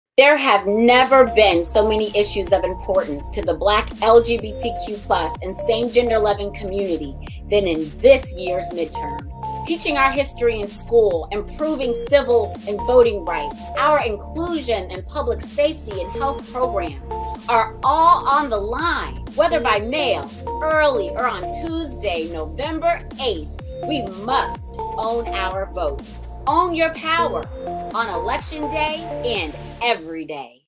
Own Your Power by Voting on or before November 8, 2022
NBJC Deputy Executive Director Victoria Kirby York discusses the importance of voting in this critical midterm election.